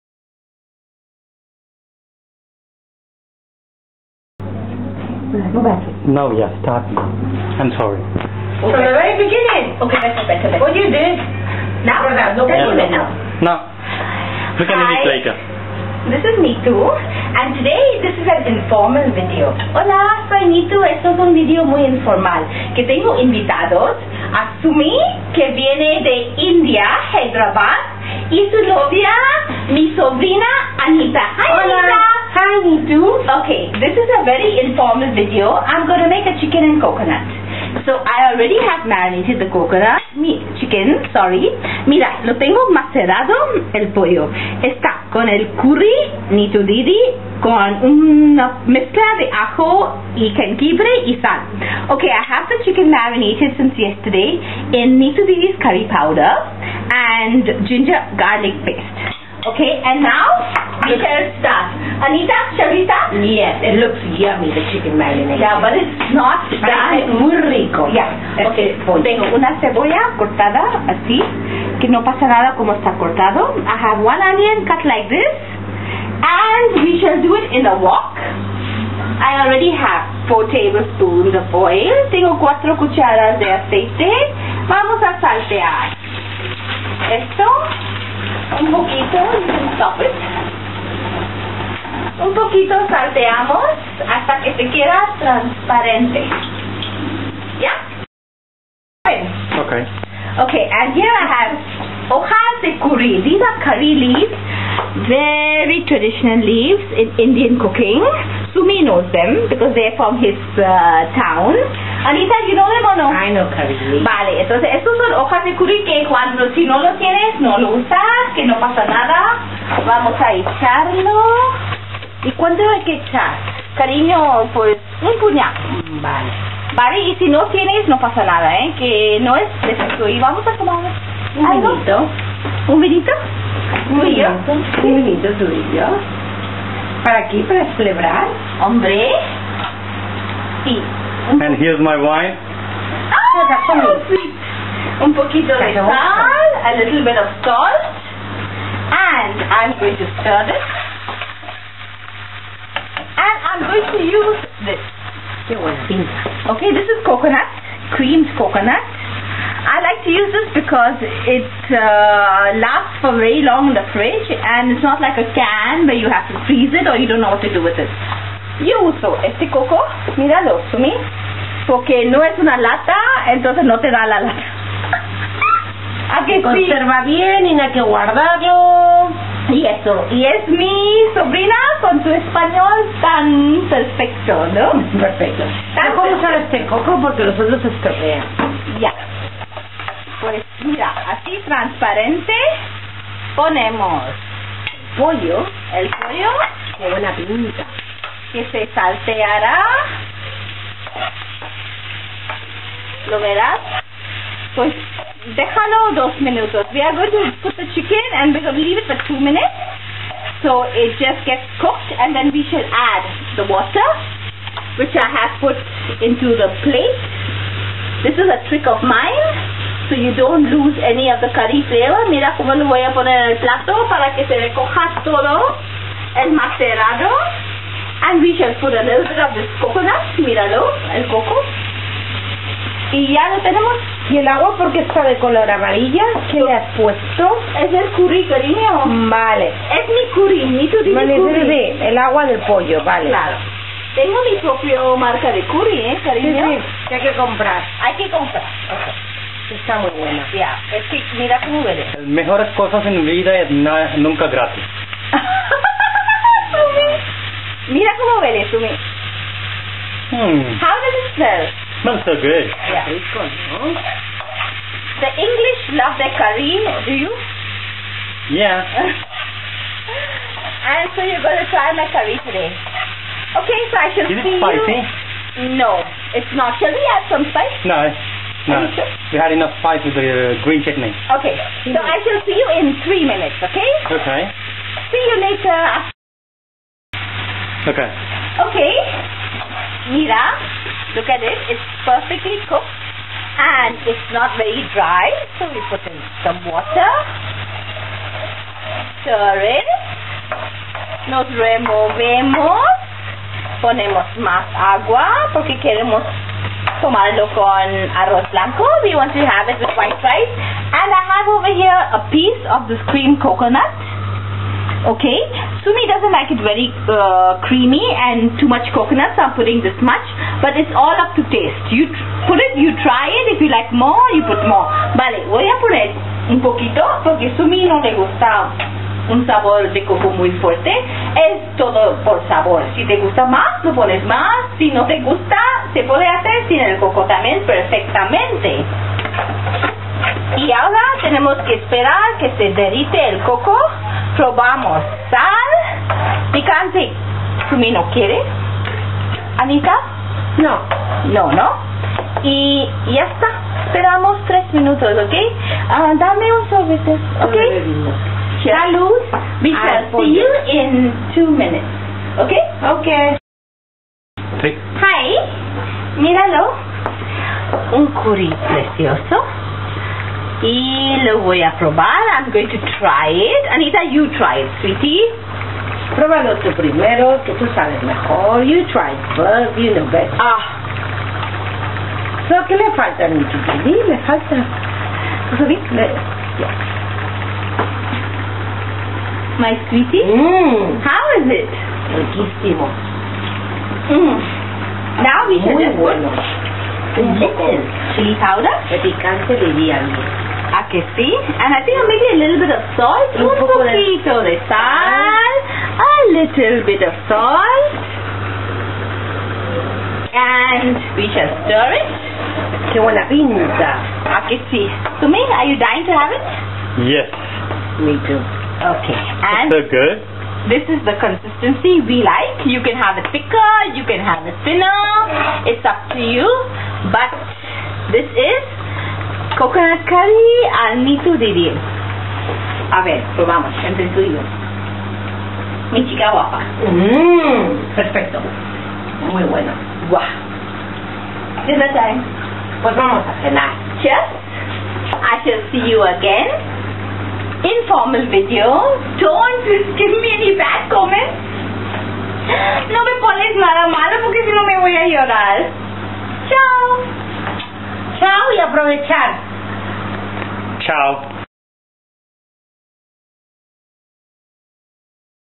CHICKEN IN COCONUT.wmv
how to make a creamy chicken curry with coconut
como hacer 1 pollo al curry con coco